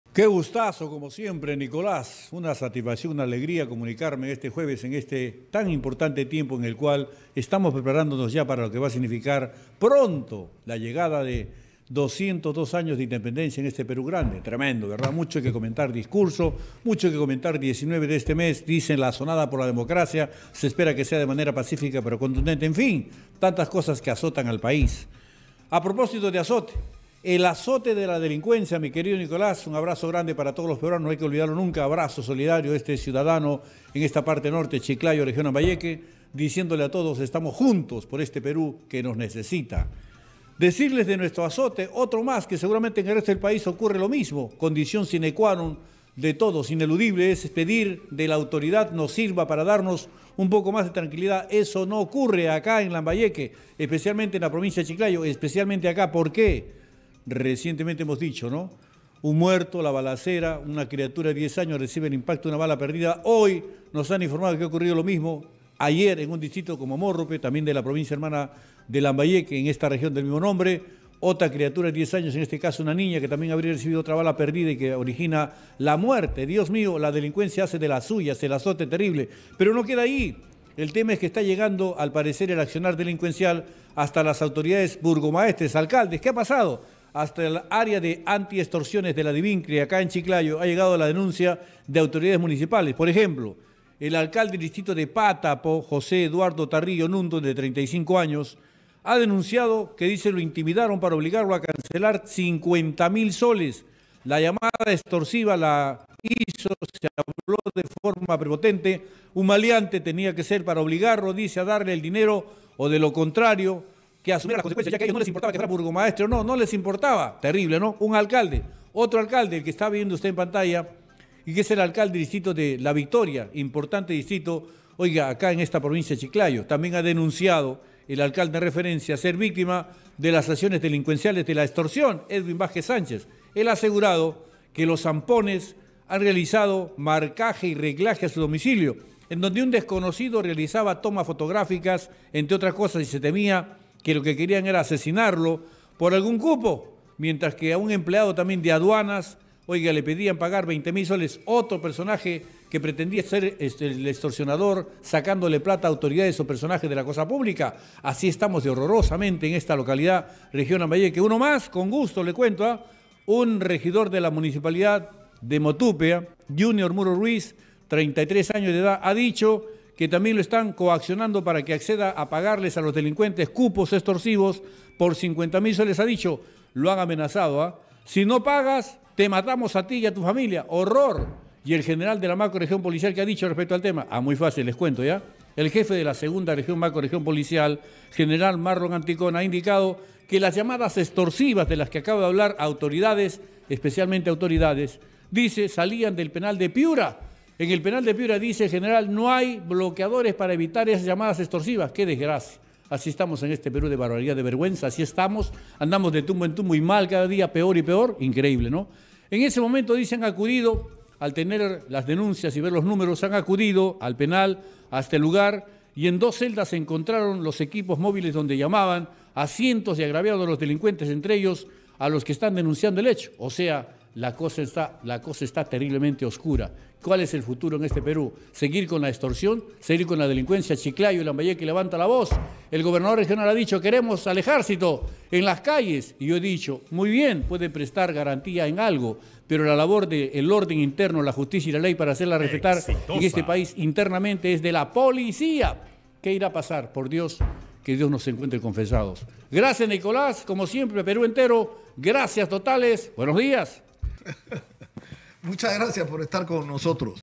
🔴🔵 Lambayeque: Extorsionadores amenazan a alcaldes para que paguen cupos
Lambayeque: El alcalde del distrito de Pátapo, José Eduardo Tarrillo, indicó que recibió amenazas de extorsionadores para que pague 50 mil soles.

Exitosa Perú
"Exitosa: La voz que integra al Perú"

Corporación Universal